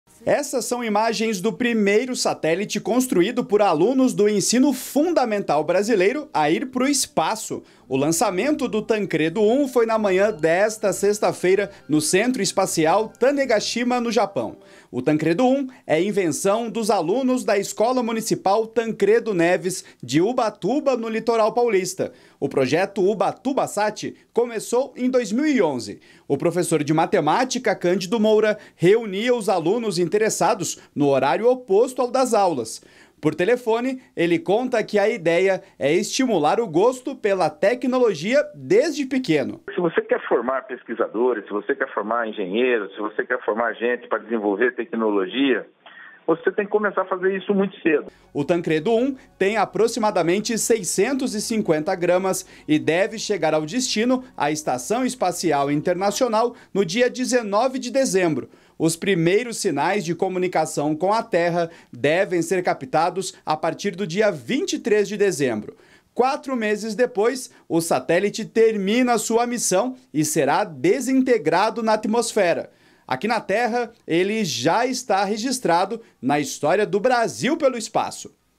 Lançado primeiro satélite construído por estudantes brasileiros do ensino médio
REPÓRTER NBR - 09.12.16: Nesta sexta-feira (9), foi lançado o primeiro satélite construído por alunos brasileiros do ensino médio. O Tancredo 1 foi desenvolvido por estudantes da Escola Municipal Tancredo de Almeida Neves, de Ubatuba (SP). Lançamento ocorreu no Centro Espacial de Tanegashima, no Japão.